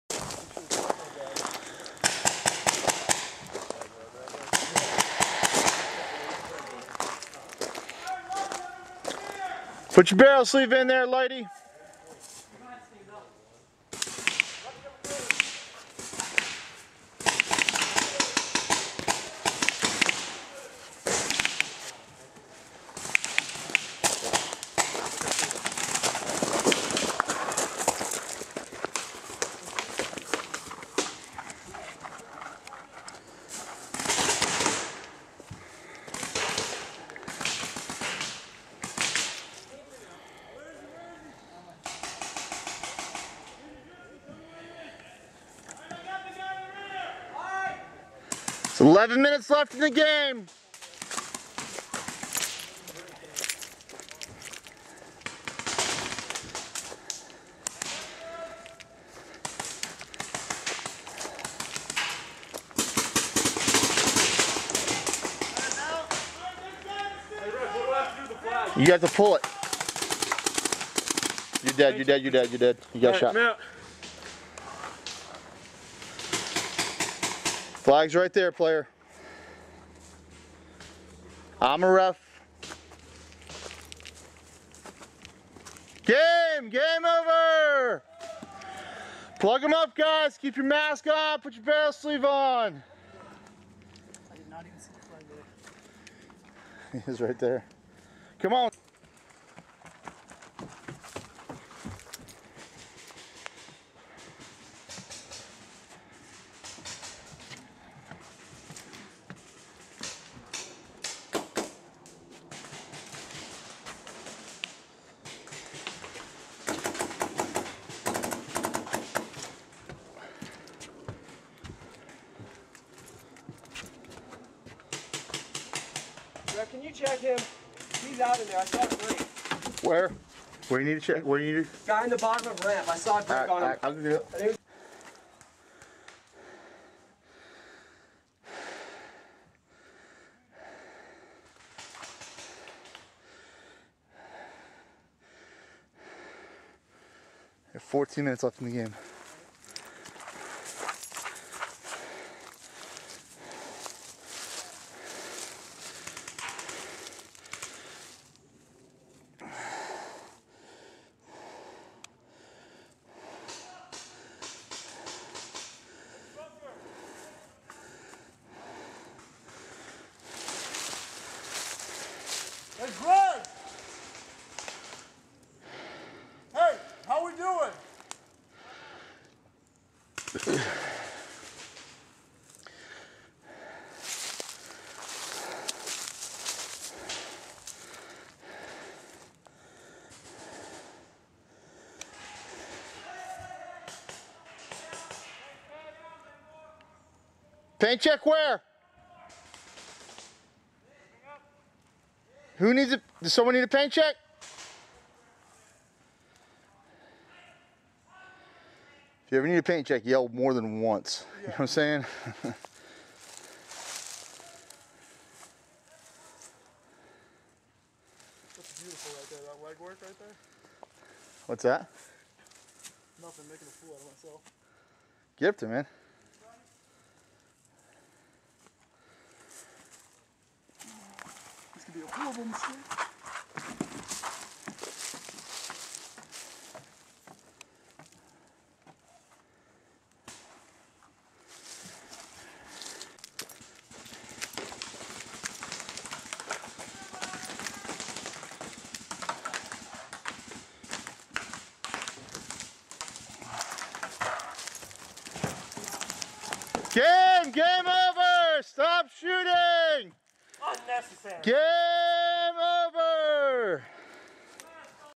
College Day
Virginia Tech, Virginia Military Institute, Washington & Lee and Hargrave Military Acedemy play paintball at Big Pearl Paintball.